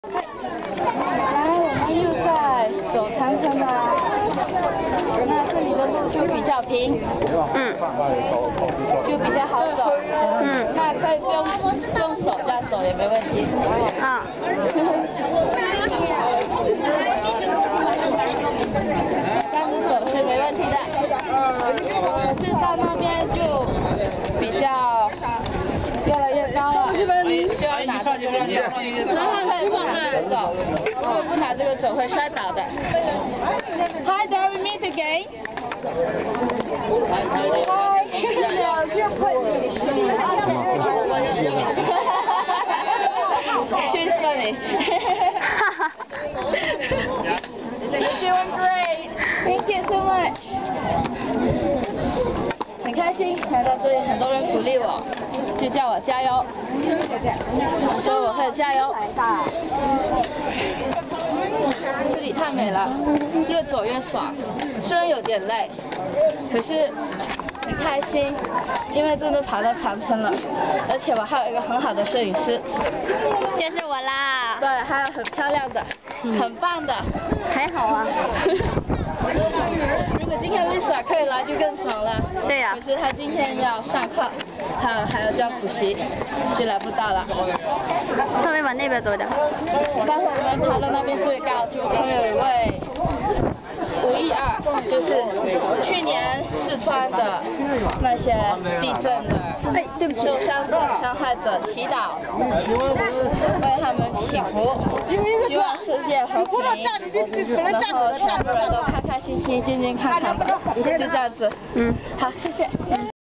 The Great Wall Challenge
A handicap girl taking up The Great Wall (Ba Da Ling) Challenge on May 12th, 2009 - Part 9